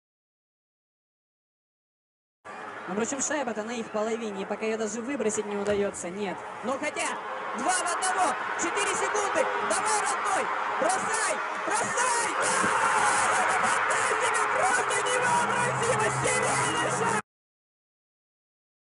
Последние за 1 секунды гол и победил Россия
Последние за 1 секунды гол и победил Россия над сша 6-5